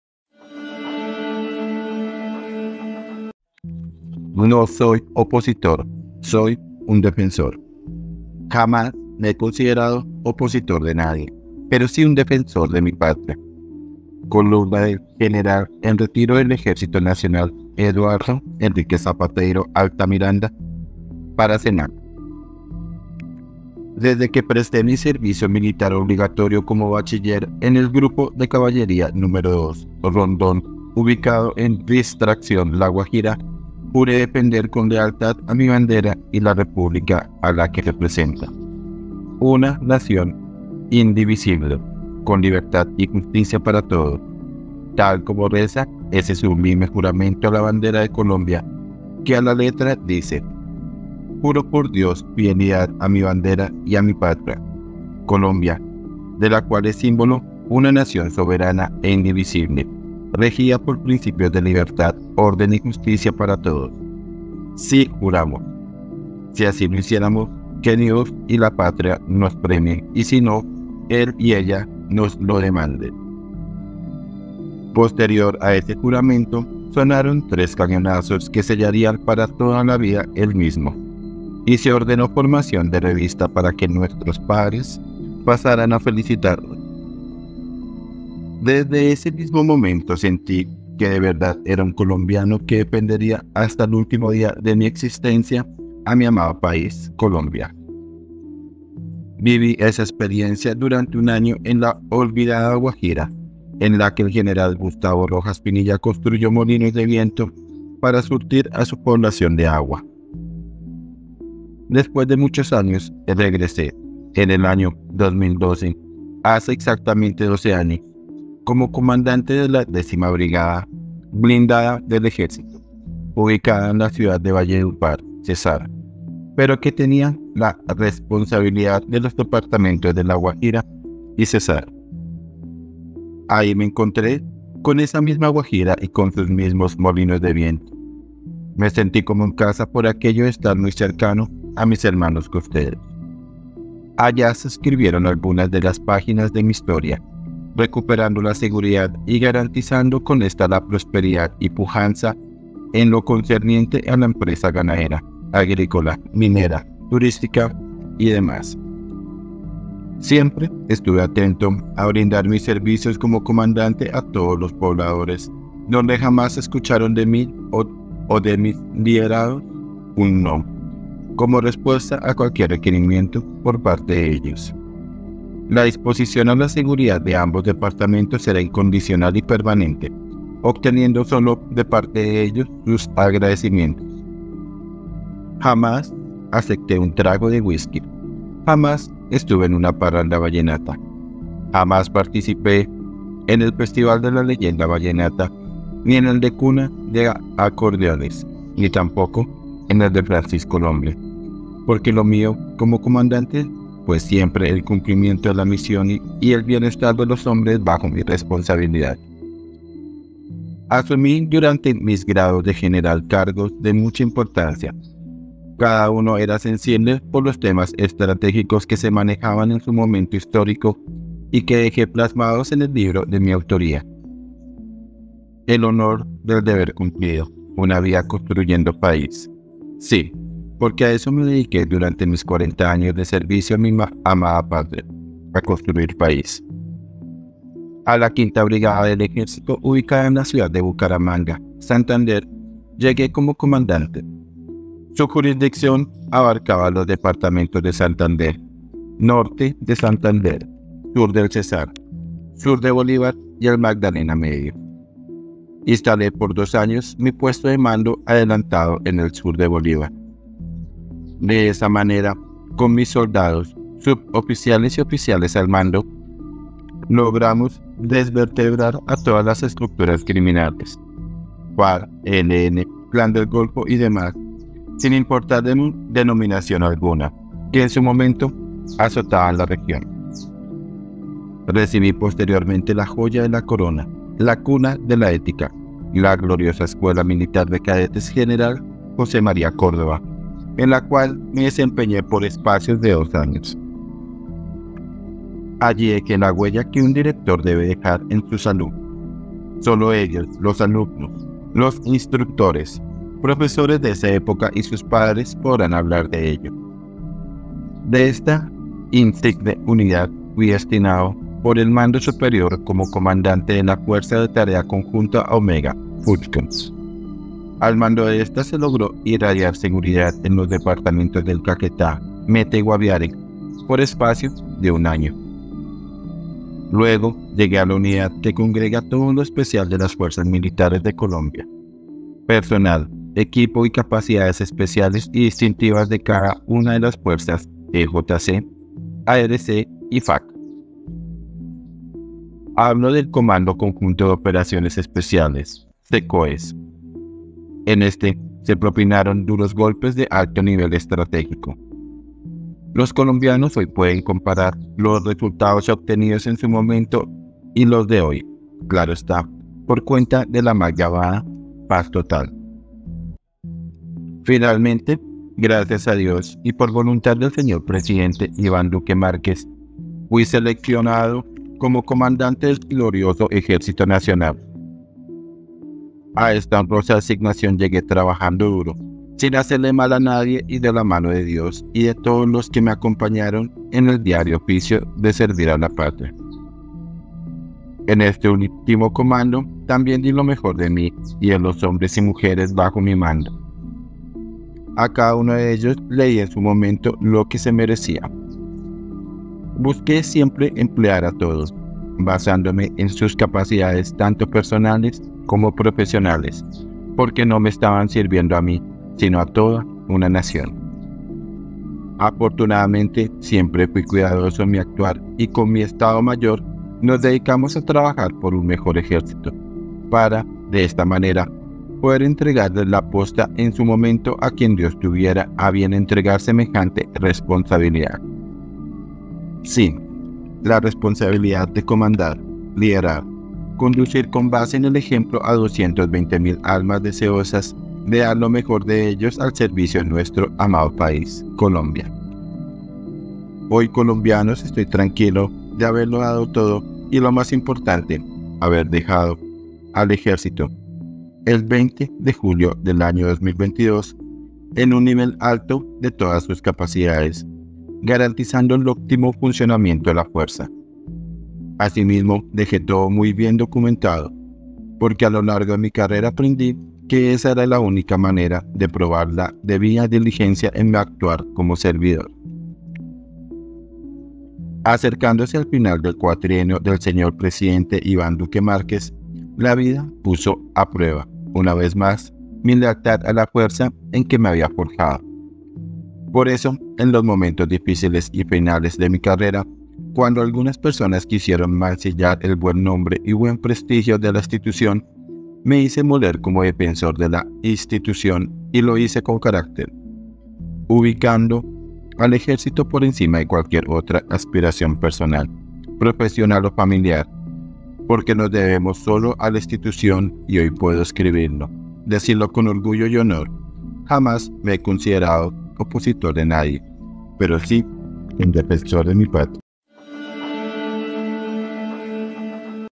NO SOY OPOSITOR, SOY UN DEFENSOR ⭕ Columna General (r) Eduardo Zapateiro
Desde que presté mi servicio militar obligatorio como bachiller, en el grupo de caballería No. 2 “Rondón”, ubicado en Distracción, La Guajira, juré defender con lealtad a mi bandera y la República a la que representa, una nación indivisible, con libertad y justicia para todos, tal como reza ese sublime juramento a la bandera de Colombia, que a la letra dice:
columna General en retiro del Ejército Nacional Eduardo Enrique Zapateiro Altamiranda
SEMANA